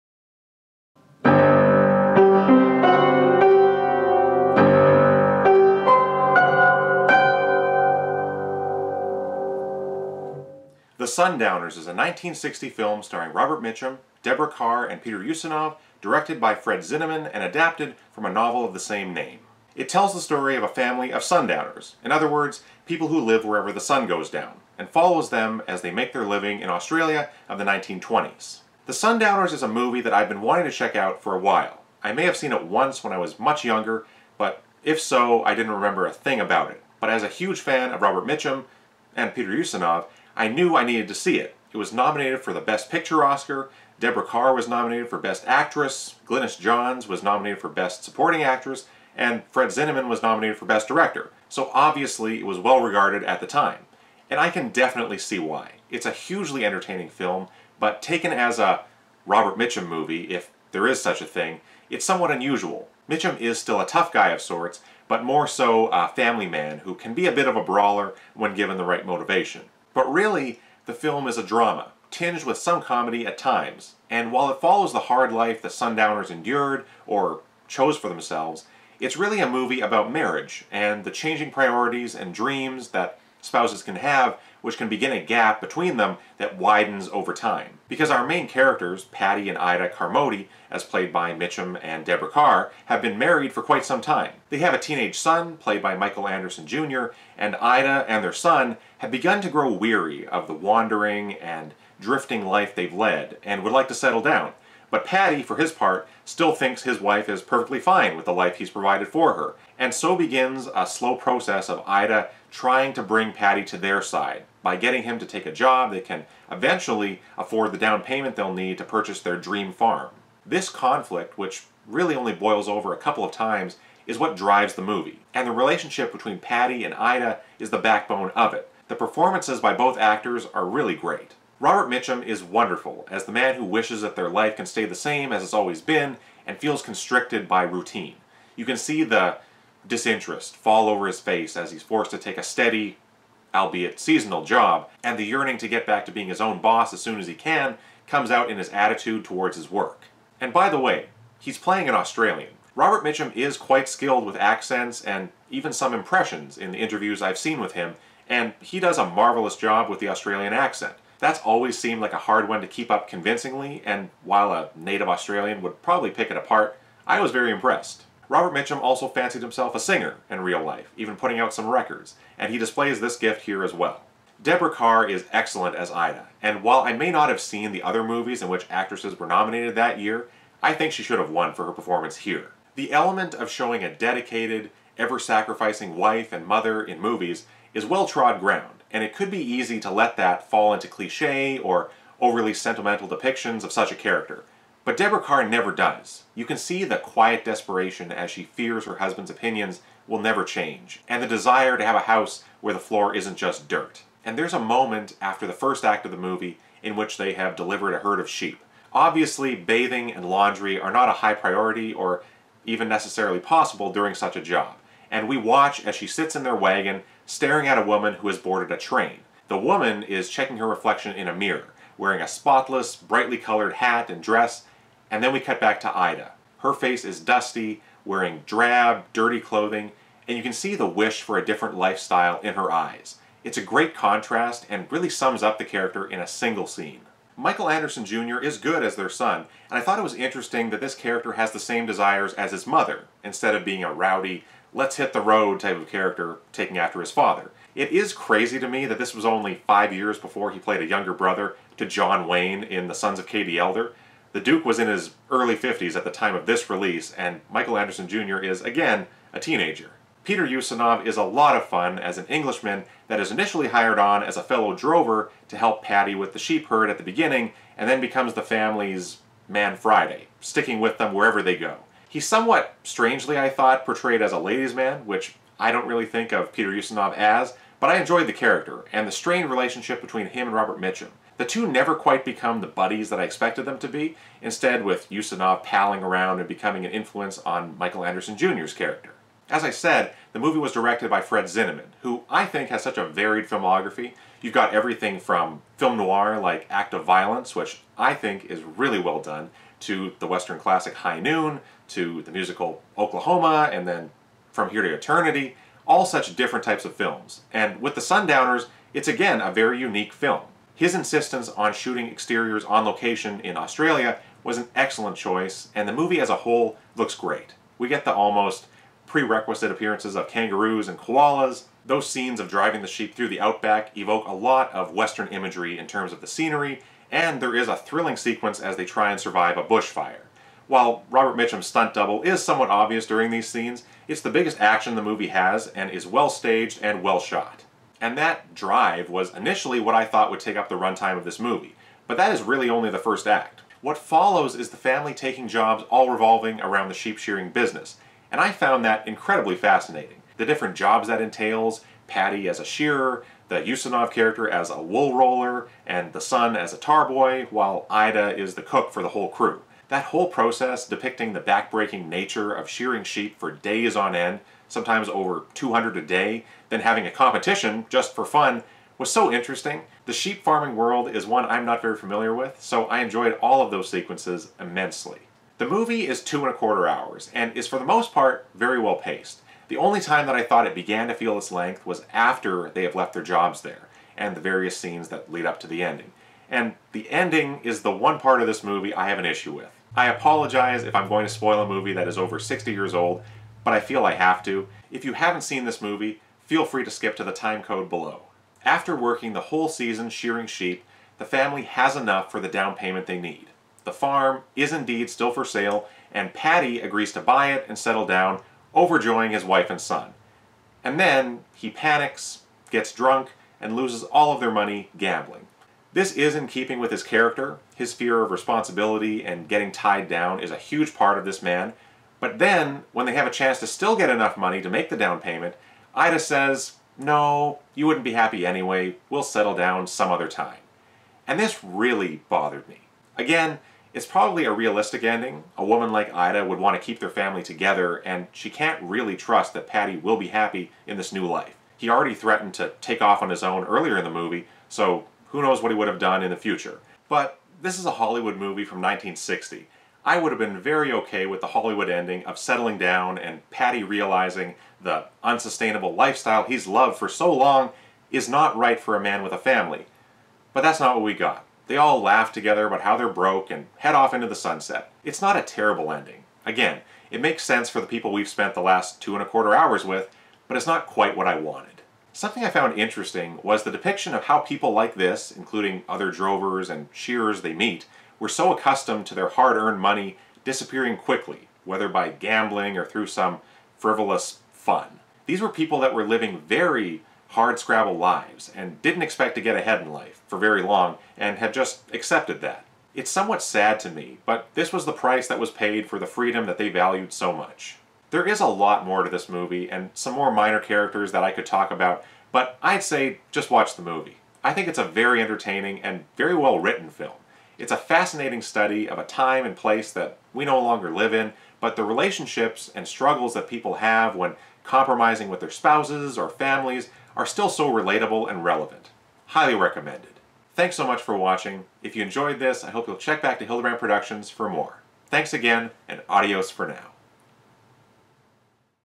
The Sundowners (1960) - Movie Review
Today I venture into the Australian outback with 'The Sundowners'! Join me as I discover this classic film with performances from some of the finest actors of the era.

Starring Robert Mitchum, Deborah Kerr and Peter Ustinov

Directed by Fred Zinnemann